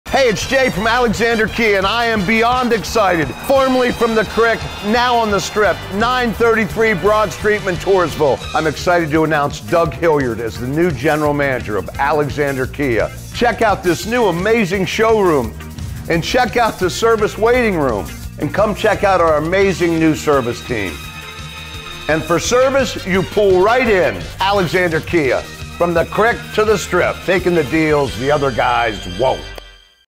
Visit Our New Location! - Blaise Alexander Kia of Montoursville
The future looks bright at Alexander Kia! We've moved to the strip in Montoursville at 933 Broad Street! Whether you're looking for no-pressure sales or top-notch service, we're ready to deliver! We can't wait for you to visit our new showroom, but you can always explore 24/7